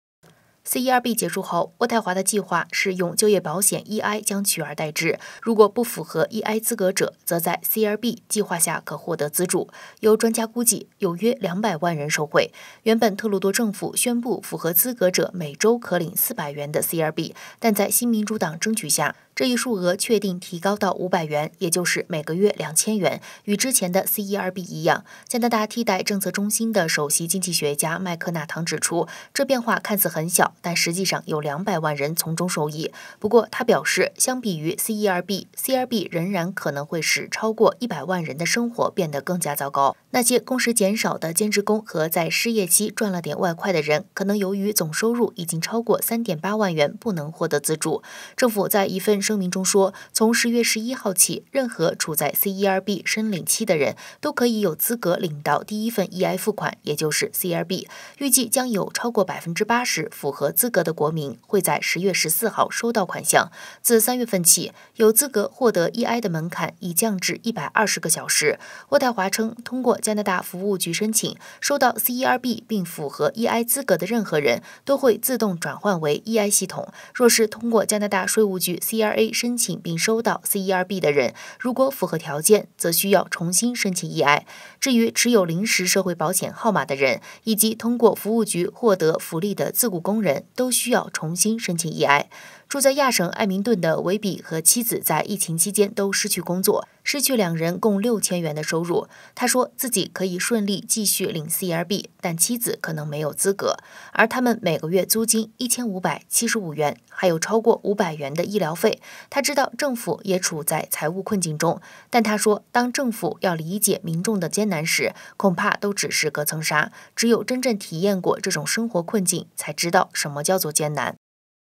CERB退出历史舞台 EI继续提供援助
CERB结束后，渥太华的计划是用就业保险(EI)将取而代之，如果不符合EI资格者则在CRB(Canada response benefit)计划下可获得资助。有专家估计，有约200万人受惠。原本特鲁多政府宣布符合资格者每周可领400元CRB，但在新民主党争取下，这一数额确定提高到500元。